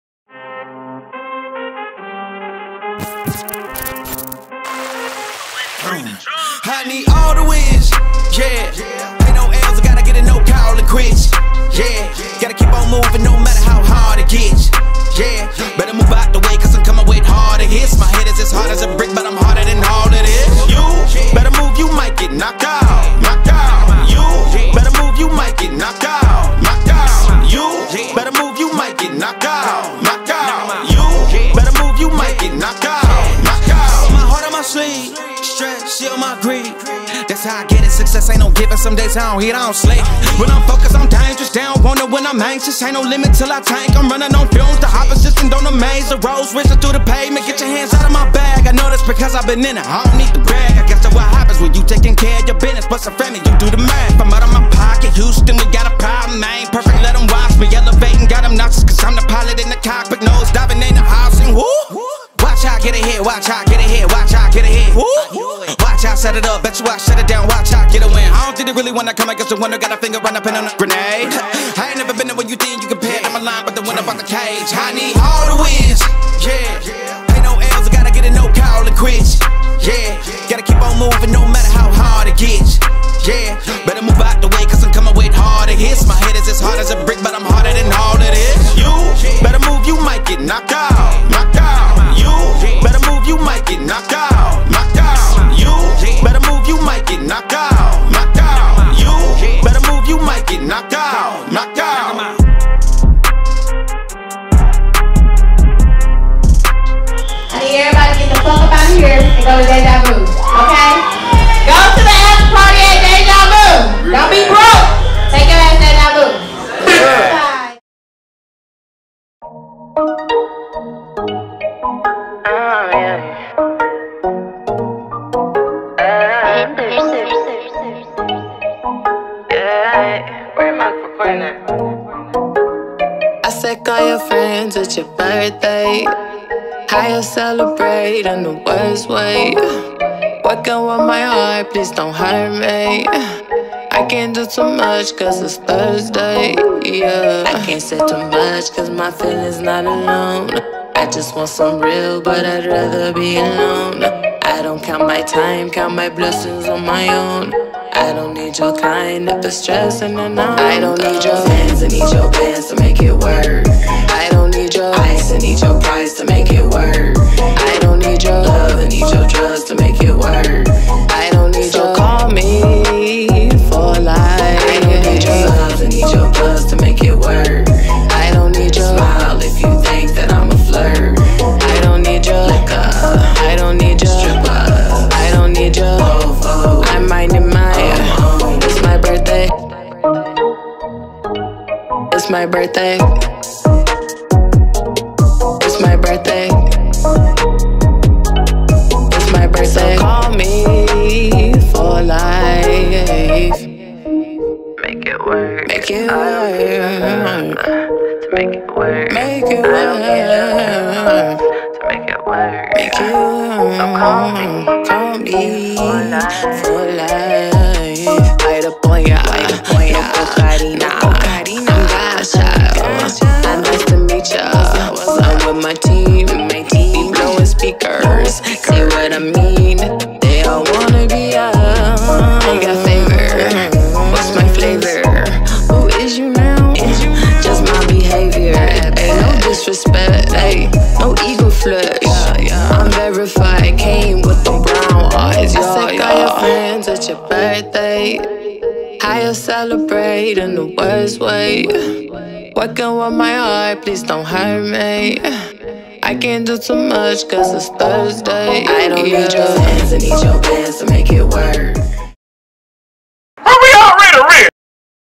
BCE Presents (NYE) BASH @ Club Chardonnay / Deja Vu (After Party) 4k
Blue Cheese Ent. Presents (NYE) BASH @ Club Chardonnay / Deja Vu
Feat: Dj Shunn Dadon And Live Performance From /Jay Sino... After Party
This Event Was Shot & Edited By @HIDNBLACKFILMZLLC